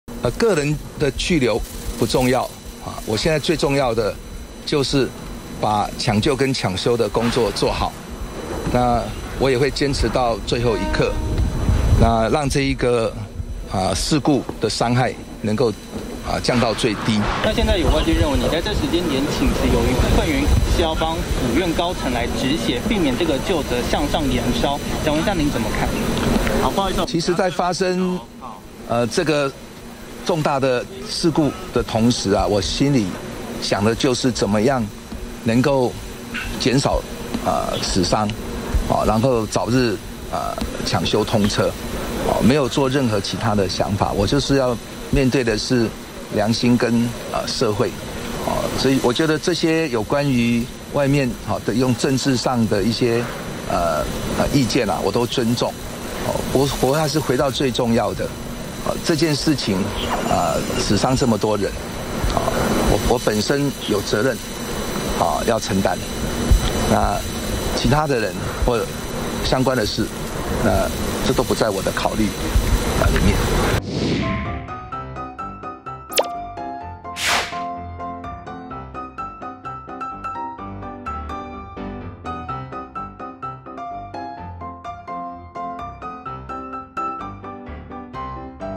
【太魯閣號出軌】為太魯閣號事故請辭 林佳龍:個人去留不重要@中天新聞CtiNews 20210405
台鐵太魯閣號發生嚴重出軌事故，造成多人不幸罹難。對此，交通部長林佳龍今天早上再次來到事故現場，說明已經向府院請辭，個人去留不重要，現在重要的是搶修工作要做好。另外，外界質疑，林佳龍這時候請辭好像在幫府院高層止血，他回應，這次死傷那麼多人，自己有責任承擔，關於政治意見，表示尊重。